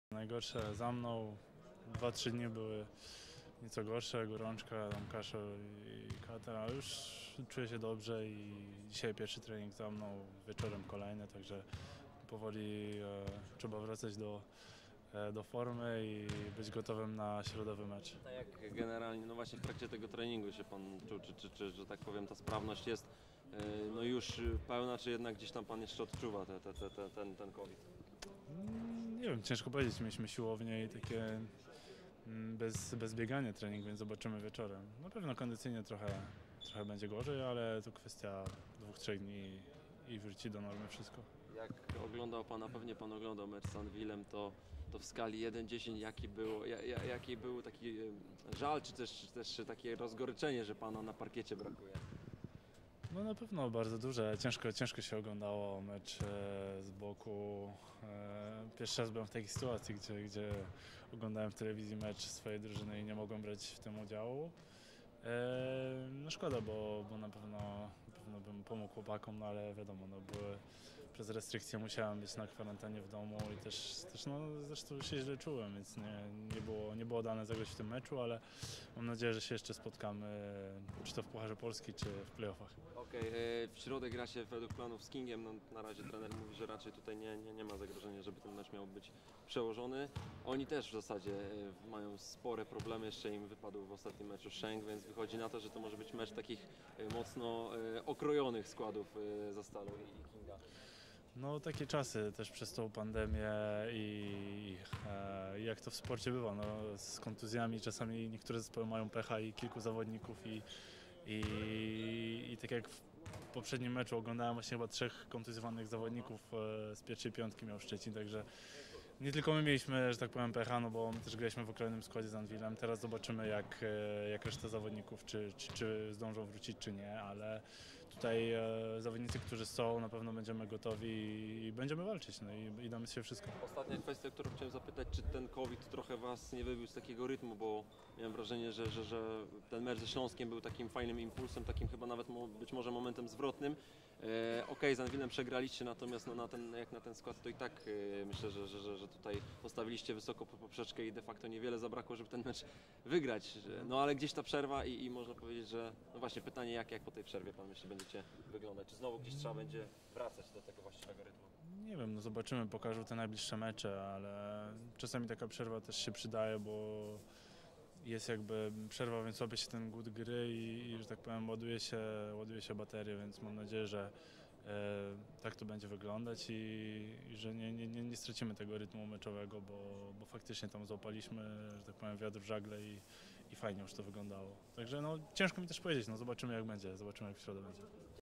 J. Zyskowski o tym, jak przeszedł Covid-19: Najgorsze już za mną - 11.01.2021r.
Jarosław Zyskowski, zawodnik Enei Zastalu BC Zielona Góra tłumaczy, jak przeszedł Covid-19. O tym, jak choroba wpłynęła na jego formę oraz jak czuł się oglądając grę kolegów przeciwko Anwilowi Włocławek.